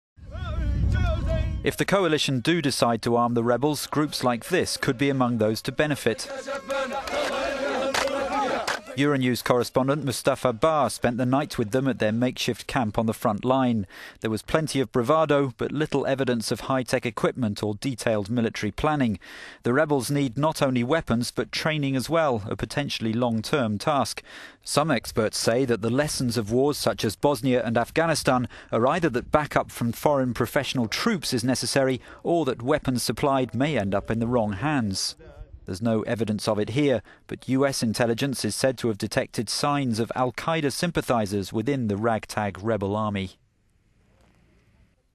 A night in the desert with the Libyan rebels
euronews correspondent Mustafa Bağ spent the night with one group of rebels at their makeshift camp on the frontline.

They sang and chanted around the campfire, cooked and smoked narghile. They slept in their cars or on mattresses on the open ground.

There was plenty of bravado - but little evidence of hi-tech equipment or detailed military planning.

If the coalition do decide to arm the rebels, groups like this could be among those to benefit.

The rebels need not only weapons but training as well - a potentially long-term task.